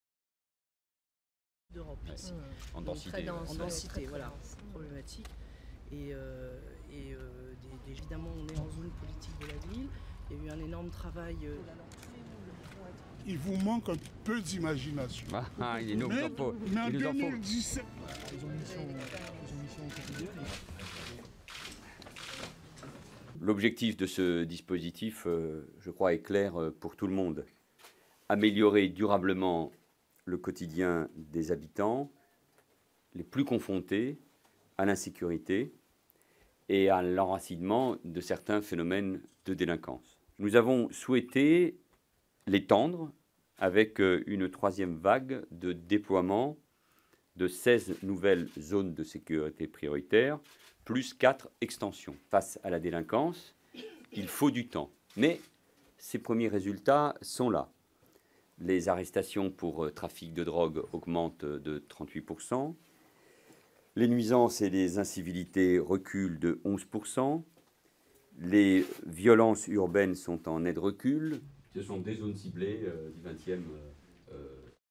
Paris: Valls annonce de la création d'une ZSP dans le XXe
Le ministre de l'Intérieur Manuel Valls a effectué ce samedi un déplacement dans le 20ème arrondissement de Paris dans le cadre de l'annonce de la création de la ZSP. Il a annoncé une aide financière de 100 000 euros et l'affectation de 42 gardiens de la paix sur ce secteur. Durée: 01:15